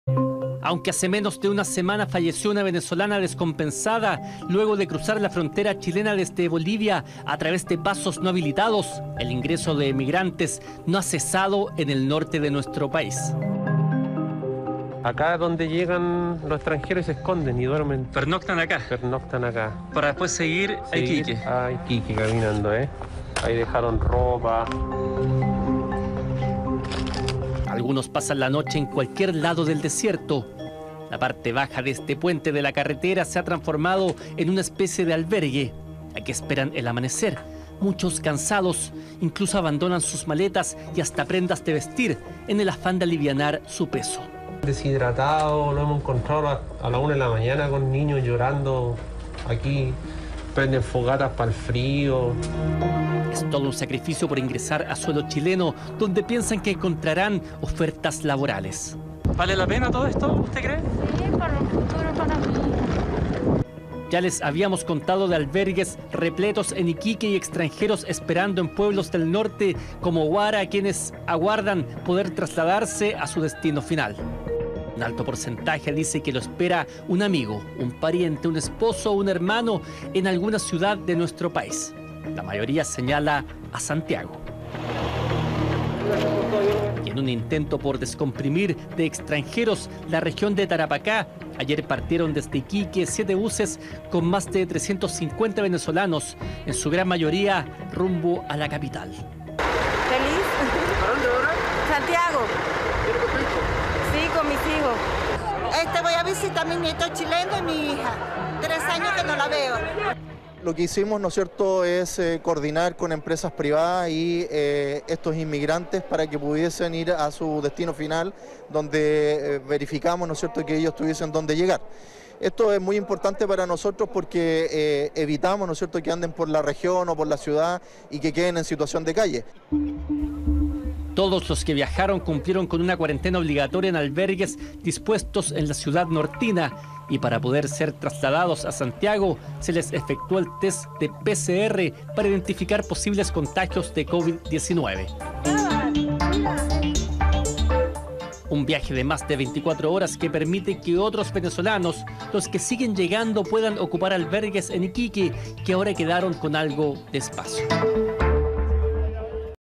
🛑 Miles de venezolanos salen del Perú para llegar a Santiago - Chile
Más de 350 ciudadanos venezolanos fueron trasladados en buses de Iquique a Santiago. Mientras, en el norte del país, la llegada de inmigrantes que cruzan pasos ilegales, no se detiene.